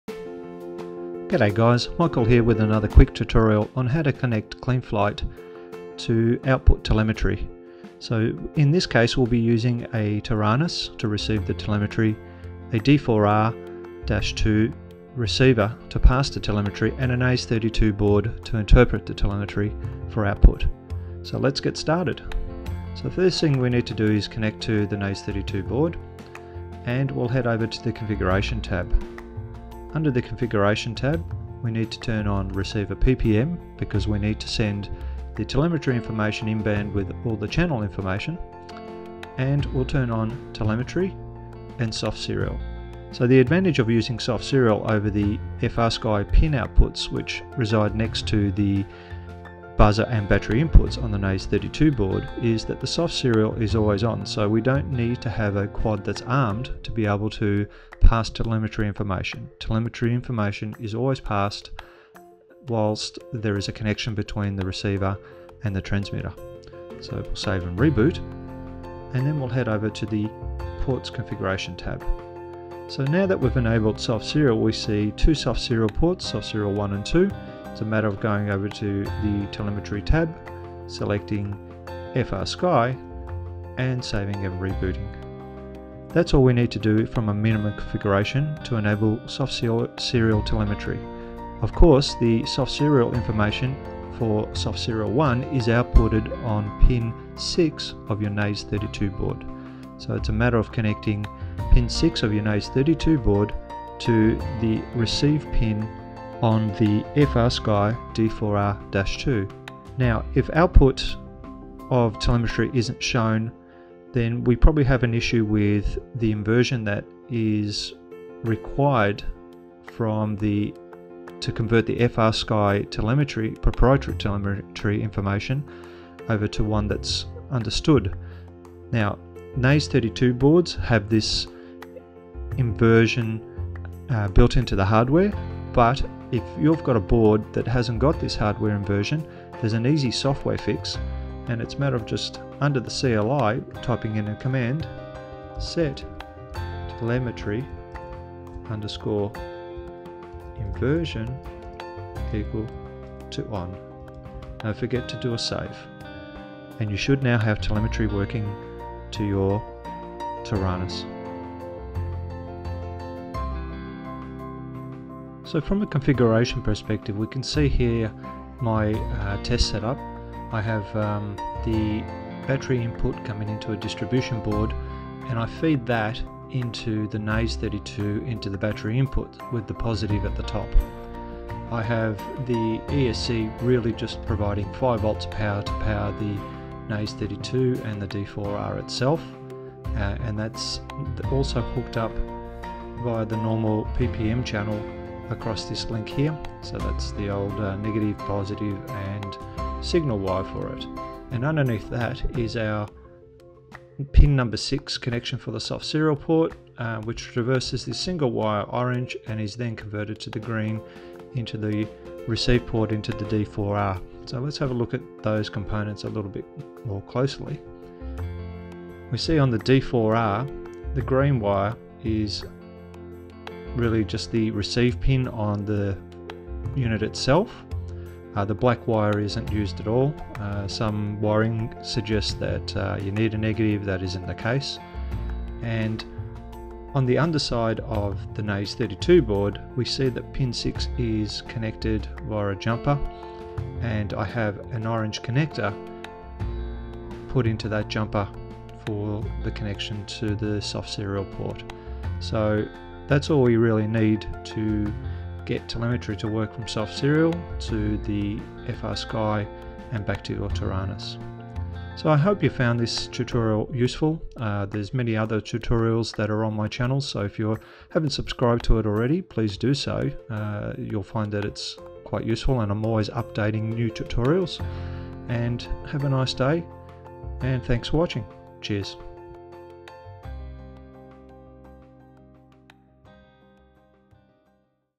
Quick Tutorial - Battery Telemetry - Softserial Configuration
In this tutorial I look at how to enable the softserial output for delivering telemetry to a remote control such as a Taranis. The difference with using softserial instead of FRSKY is that the output is always on (not when it's just armed) and is outputted on Pin 6 of the Naze32.